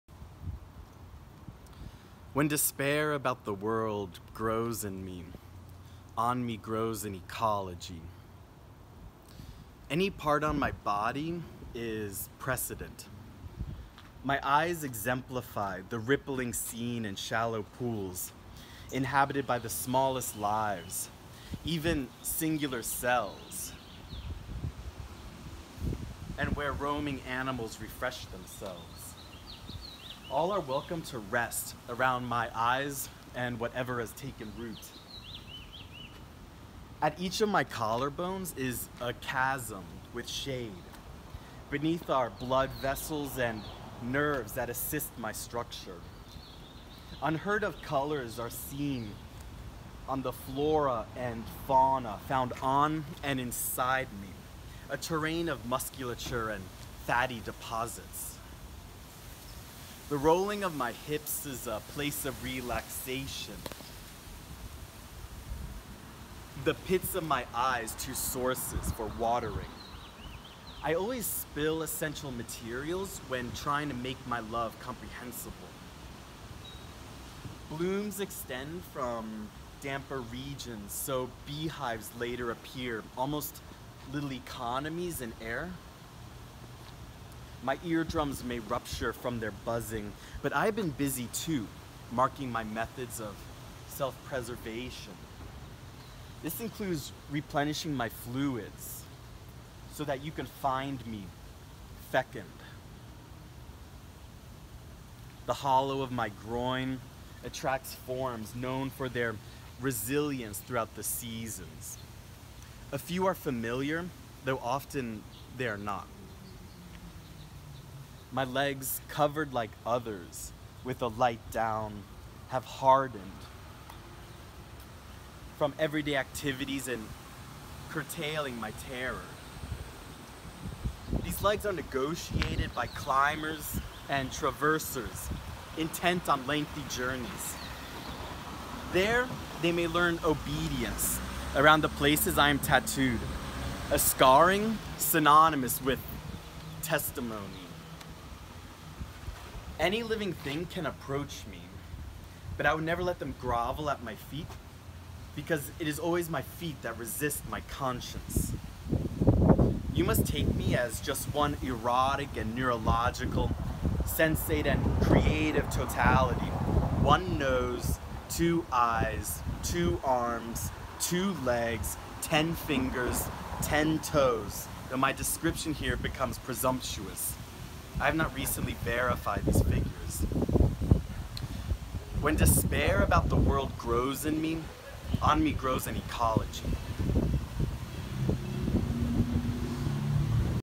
Evan Kennedy: "When despair about the world grows in me..."
Evan Kennedy is a poet, bicyclist, and resident of San Francisco, California. He is the author of The Sissies (Futurepoem); Terra Firmament (Krupskaya); a travelogue, Jerusalem Notebook (O'clock Press); and Us Them Poems (Bookthug). A memoir, "I am, am I, to trust the joy that joy is no more or less there now than before," is forthcoming from Roof Books.

Video by Adam L. Sussman

Commissioned by Small Press Traffic for Bay Area Shorts, spring 2020